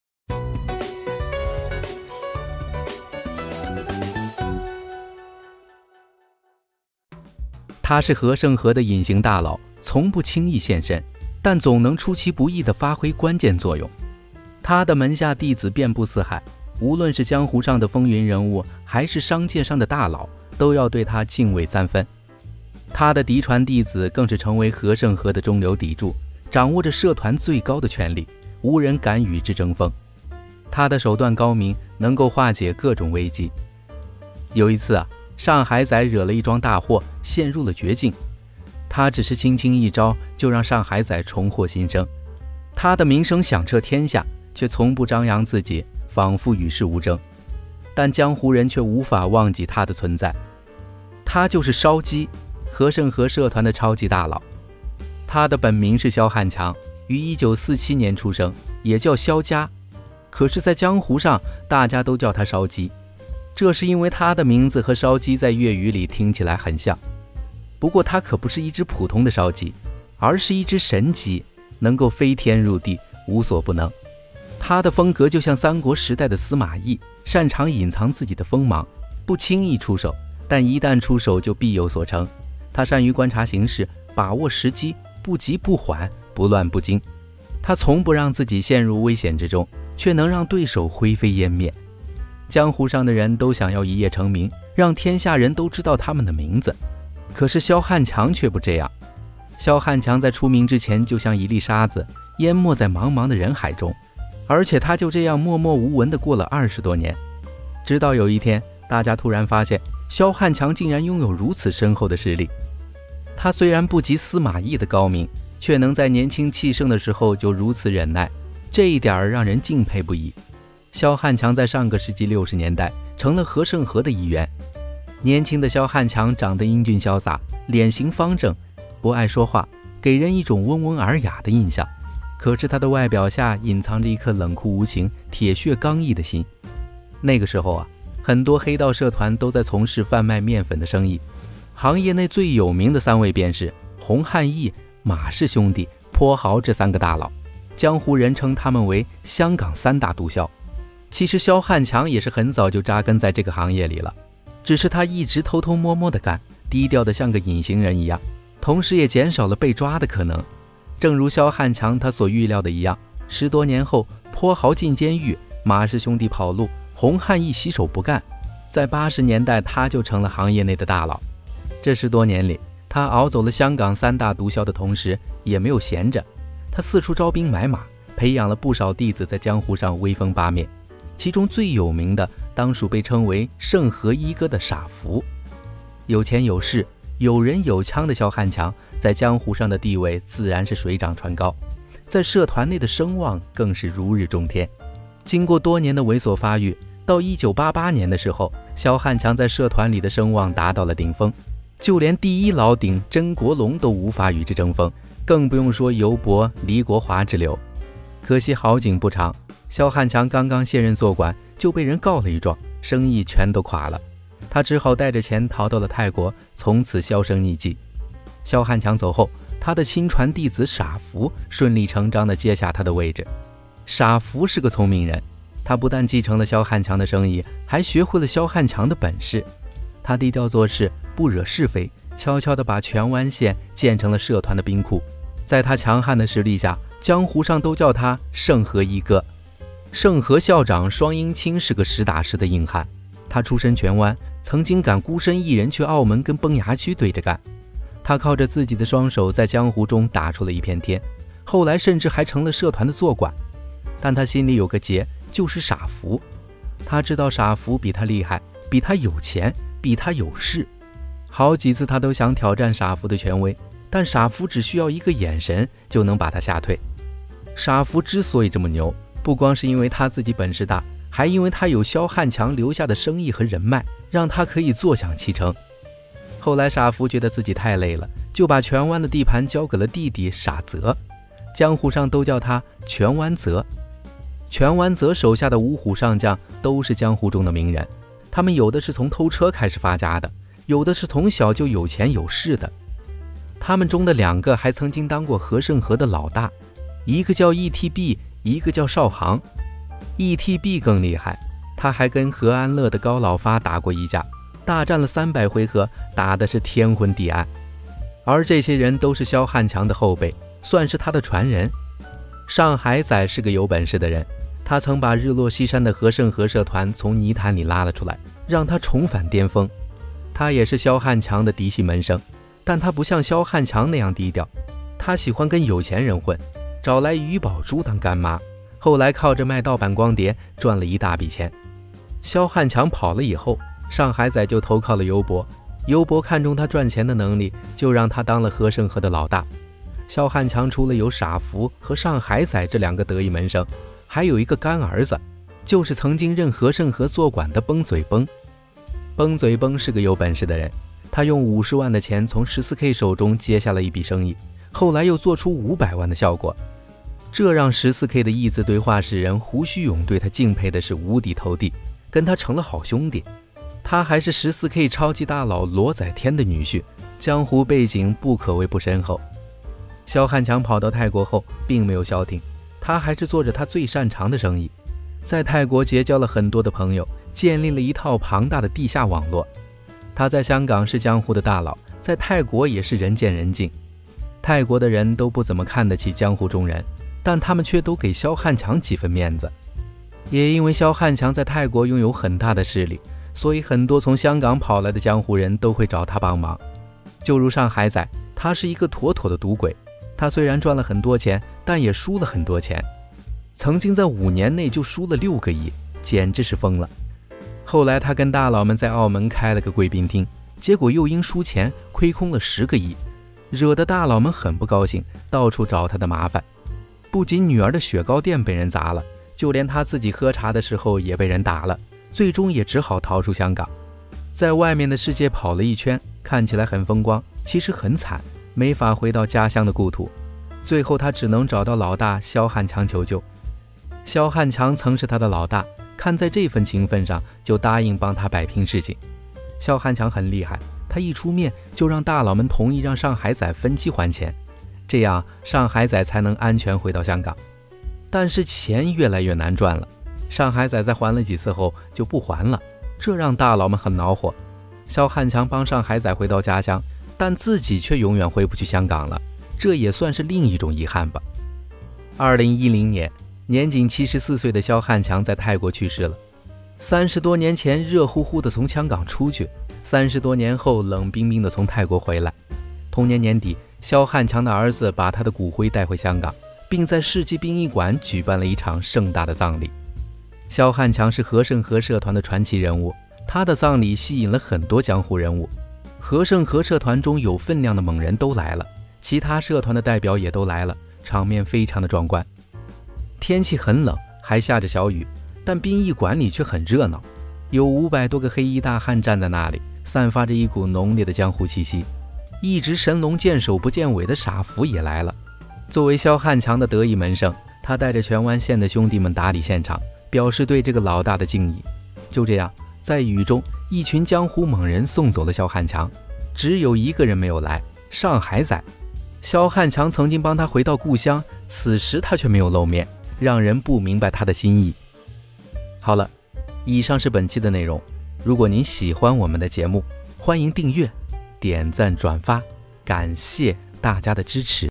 和勝和最隱秘的大佬，江湖人稱“燒雞”，不出手則已，一出手驚人︱14K︱新義安︱和勝和︱水房︱香港社團#HK娛樂驛站
和勝和最隱秘的大佬，江湖人稱“燒雞”，不出手則已，一出手驚人